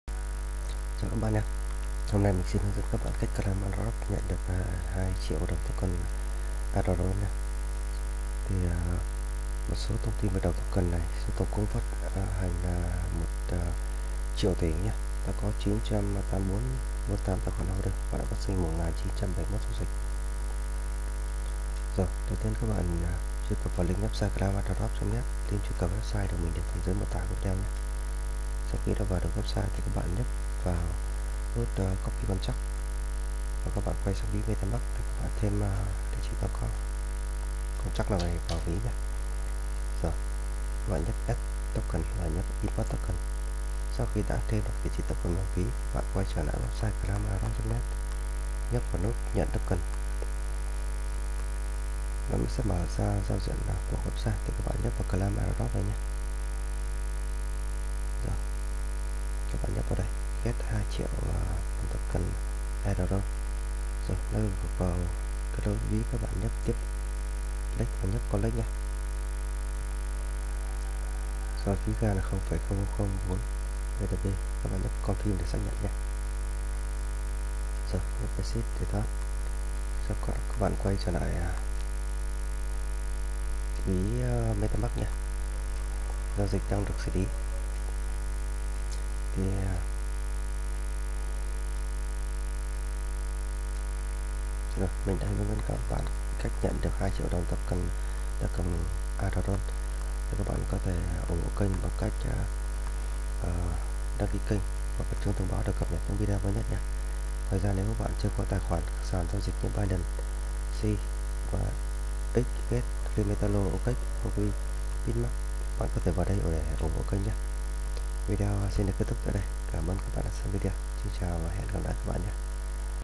I Claimed 2,000,000 ARDON $270 INSTANTLY in Trust wallet
This tutorial will show you exactly how I Claimed 2,000,000 ARDON ($270) INSTANTLY in Trust wallet.

Network : Smart Chain (BEP20)

Listing on Pancakeswap, Huobi Global and Hotbit

-Token Name: ARDON TOKEN

-Contract Address: 0xf1CdCf6d5B701d49A3Ba18C4d9227702Ee58CE03

-Symbol / Ticker:  ARDR

-Decimal: 18